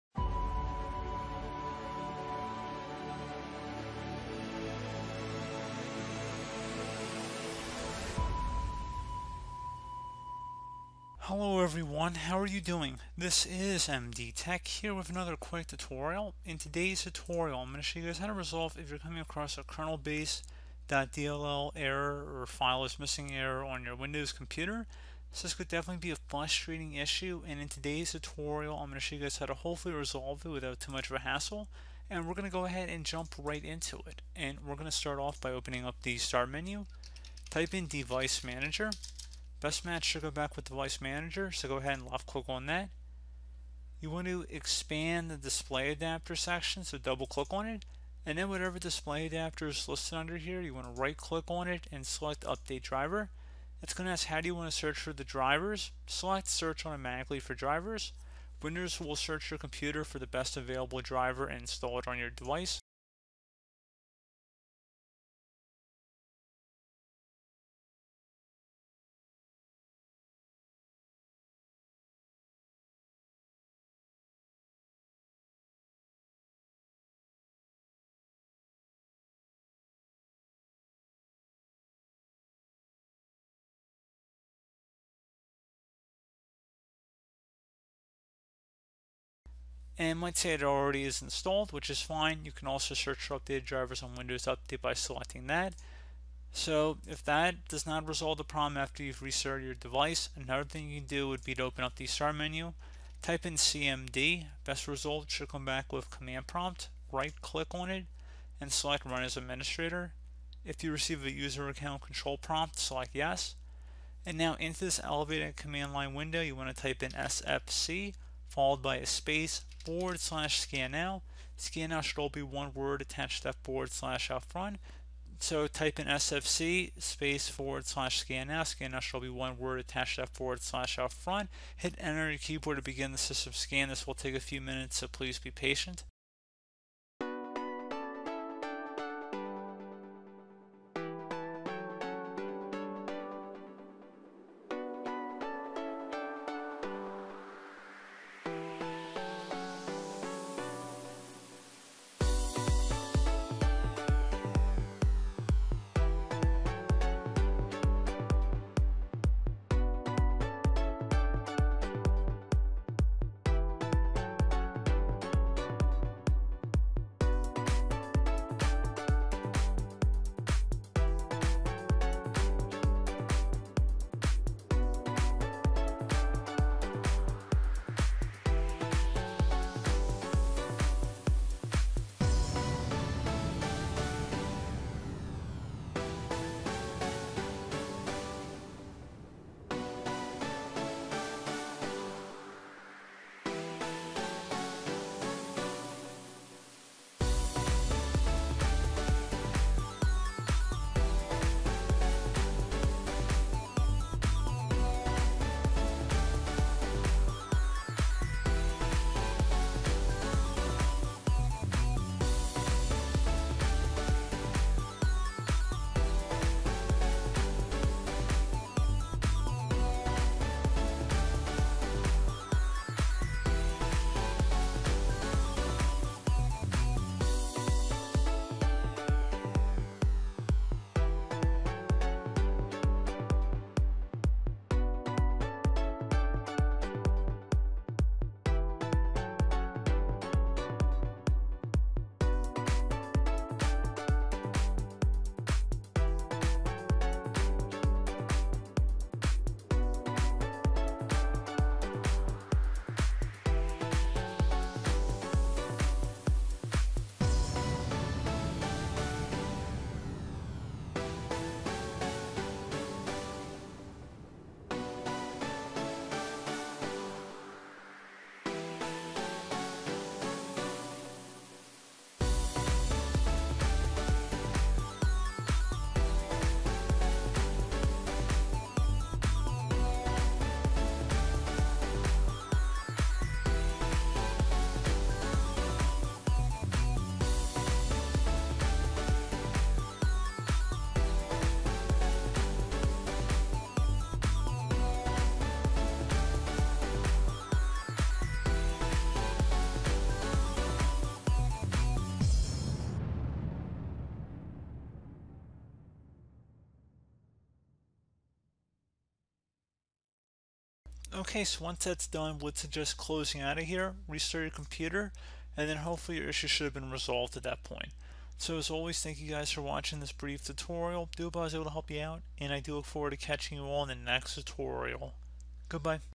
Kernelbase.Dll Fix Windows 7/8/10 | How to Fix Kernelbase.Dll Error Windows 10 Appcrash Error
Kernelbase.dll errors are caused by situations that lead to the removal or corruption of the kernelbase DLL file.

In some cases, kernelbase.dll errors could indicate a registry problem, a malware issue, like a virus infection, or even a hardware failure.

There are several ways that kernelbase.dll error messages might be phrased depending on the cause of the problem. Here are some common kernelbase.dll errors: